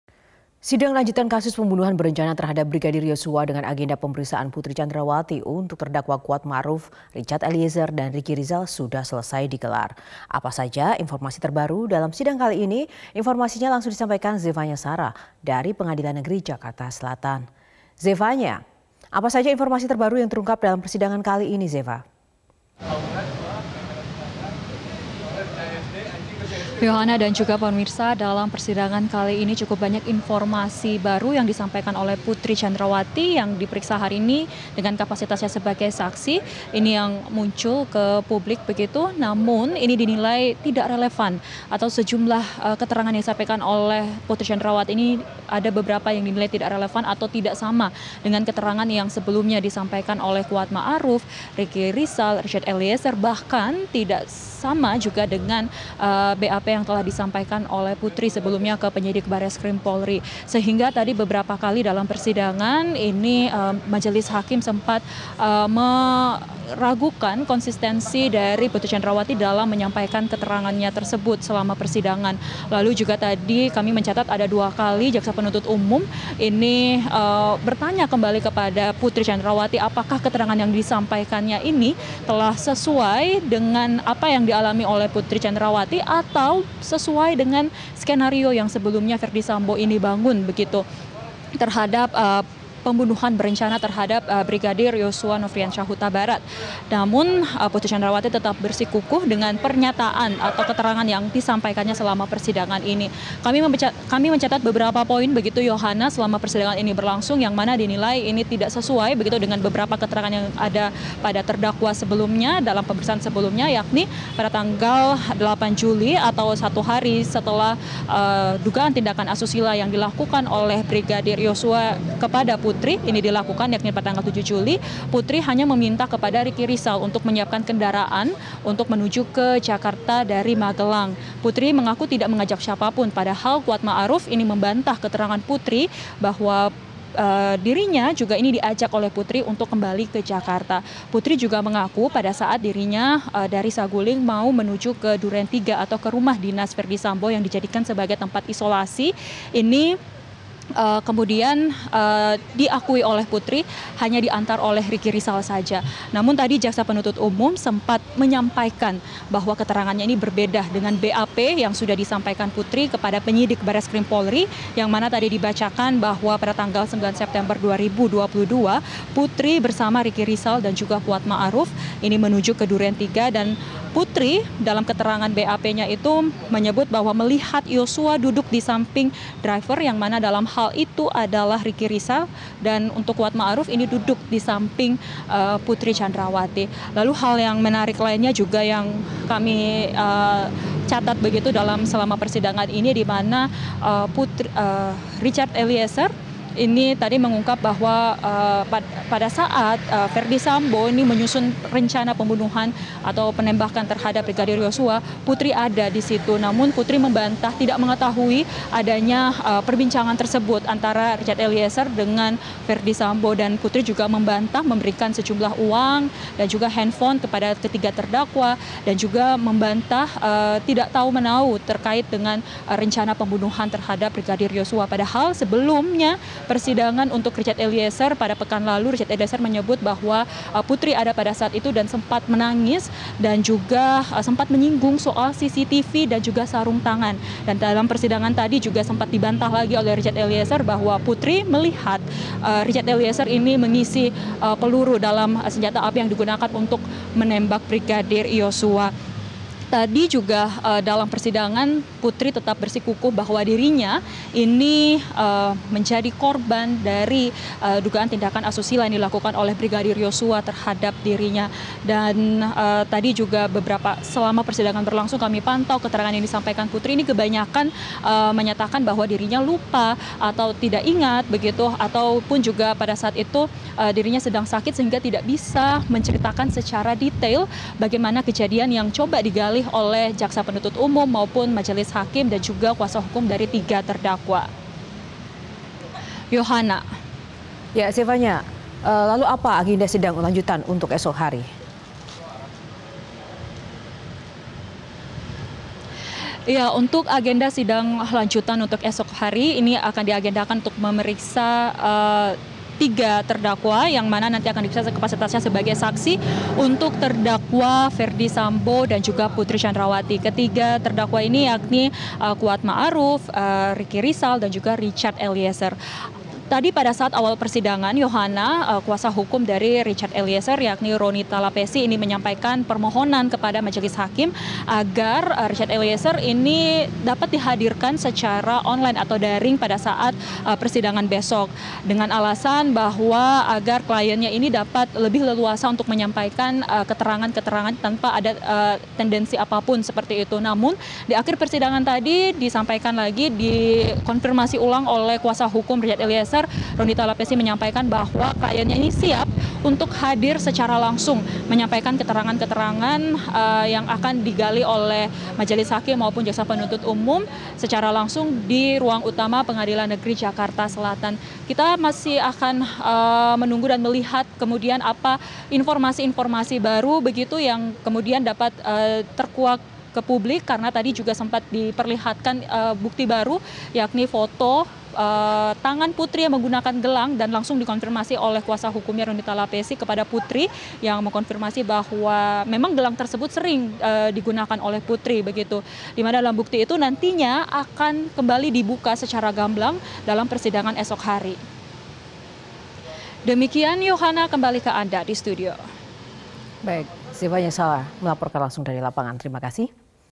Keterangan Putri Candrawathi Dinilai Tidak Relevan dengan BAP
MetroTV, Putri Candrawathi menjadi saksi dalam sidang pembunuhan berencana terhadap Brigadir J dengan terdakwa Richard Eliezer, Kuat Ma'ruf dan Ricky Rizal, Senin (12/12/2022). Keterangan Putri Candrawathi dinilai tidak relevan dengan terdakwa lain dan BAP.

Sebelumnya Putri Candrawathi mengatakan bahwa dirinya pergi ke Duren Tiga untuk isoman diantar oleh Ricky Rizal. Namun, JPU mengatakan bahwa keterangan Putri berbeda dengan BAP yang sudah disampaikan. Dalam BAP disebutkan bahwa Putri bersama Ricky Rizal dan Kuat Ma'ruf menuju ke Duren Tiga.

JPU sempat bertanya dua kali kepada Putri apakah keterangan yang disampaikannya ini telah sesuai dengan yang dialaminya atau sesuai dengan skenario Ferdy Sambo. Namun, Putri Candrawathi tetap bersikukuh dengan keterangan yang disampaikan selama persidangan.